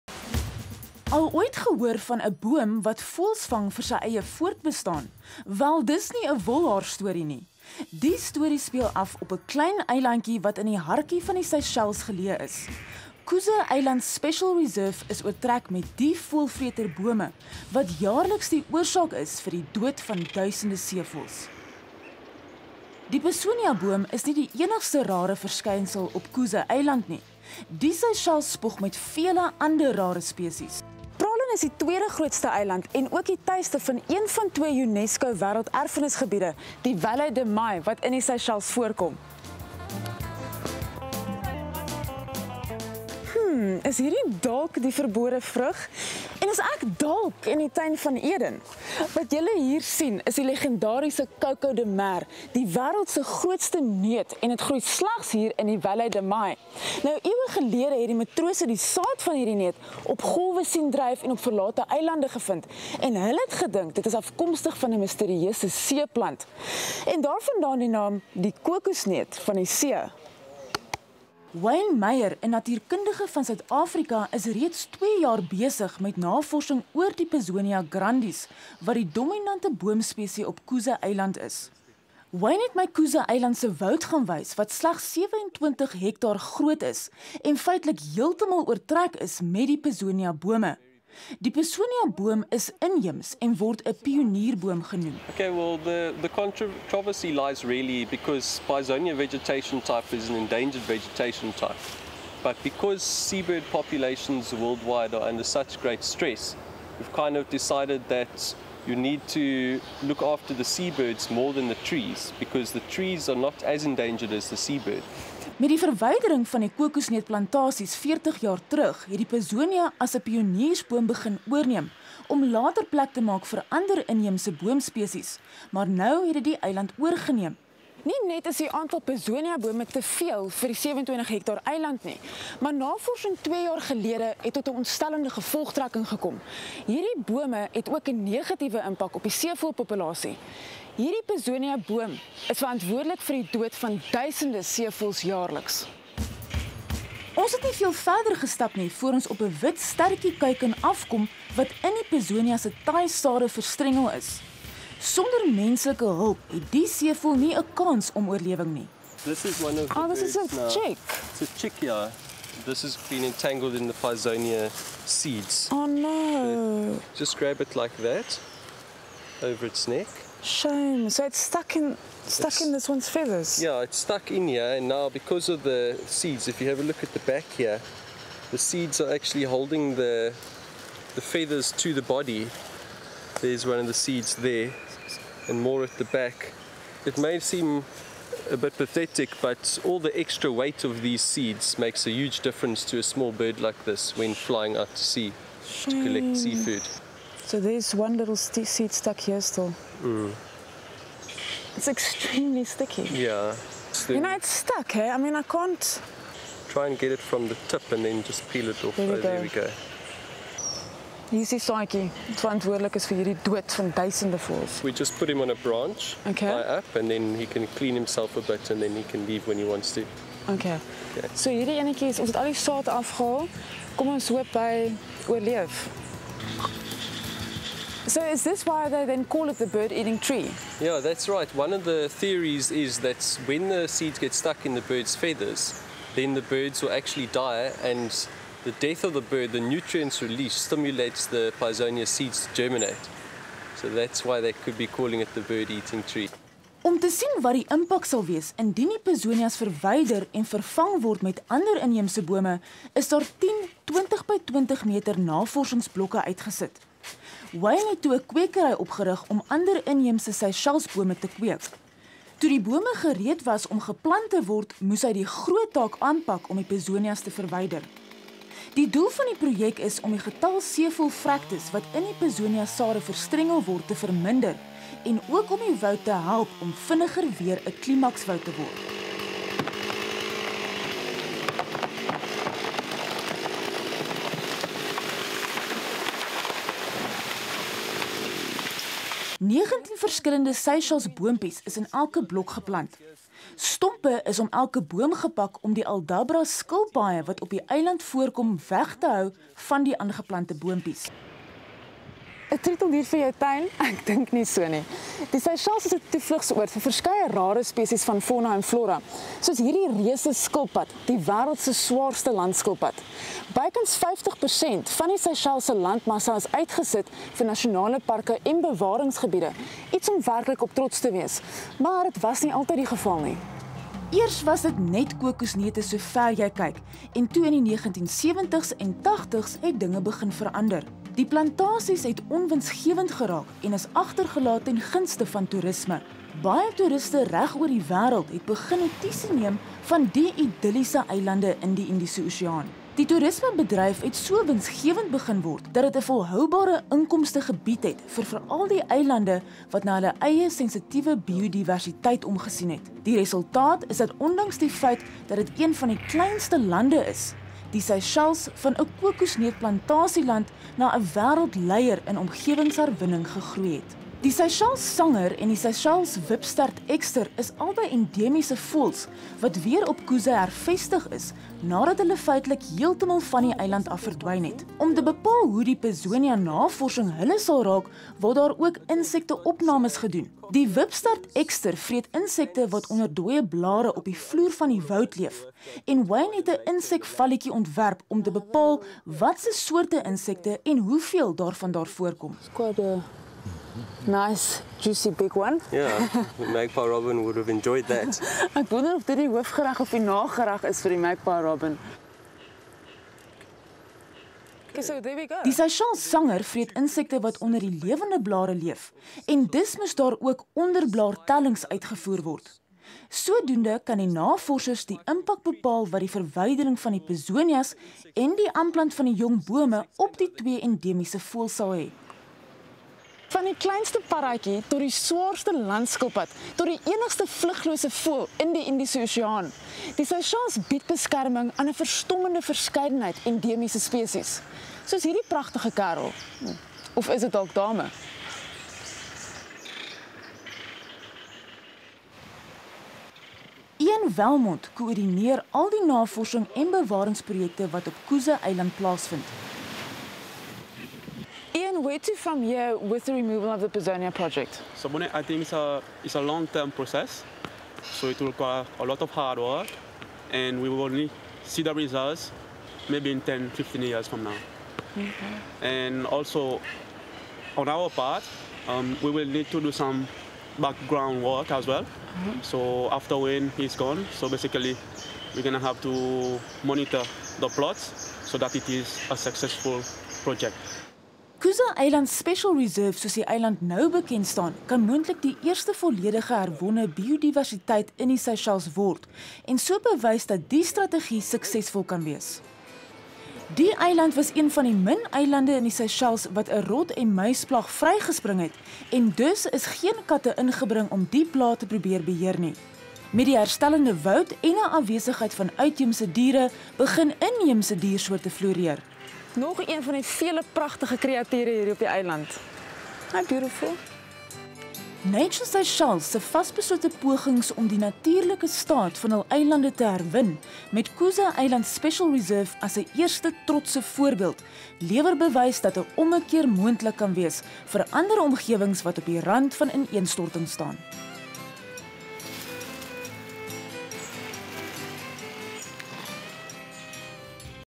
Pisonia - Bird Eating Trees.mp4
Bird-Eating Trees: Seychelles's Cousin Island, all of 27 hectares, has been on the forefront of conservation projects by scientists around the world for a number of years. Until the 1970's Cousin Island had been largely converted to a coconut plantation. This meant the removal of most indigenous vegetation which had long-lasting impacts on the island. Forty years down-the-line, Nature Seychelles has done much work to restore the island back to its original state. However, it can take hundreds of years for the forest reach maturity. Pisonia, the dominant tree species on the island, is indigenous to the island but its current abundance is much higher than it would have been in a climax forest. This is because Pisonia is a pioneer species, a plant that takes over disturbed areas, like recovering coconut plantations and then gives way to other species, ultimately leading to climax forest with mixed species. Last year, research on Cousin Island highlighted the fact that Pisonia is having a significant negative impact on the seabird populations of the island. This is because the tree produces very sticky fruits which get stuck in the sea birds' feathers and without human assistance this usually leads to their death. Seabird populations worldwide are currently under massive pressure for a number of reasons so Nature Seychelles has taken the decision to set up a research project, which will investigate the impacts of removing the Pisonia trees in research blocks and replacing them with other indigenous trees. This will hopefully decrease the number of birds dying from being caught in Pisonia seeds while increasing the biodiversity of the forest. Wayne Meyer, a Nature Conservator from South Africa, has been employed by Nature Seychelles to conduct the project.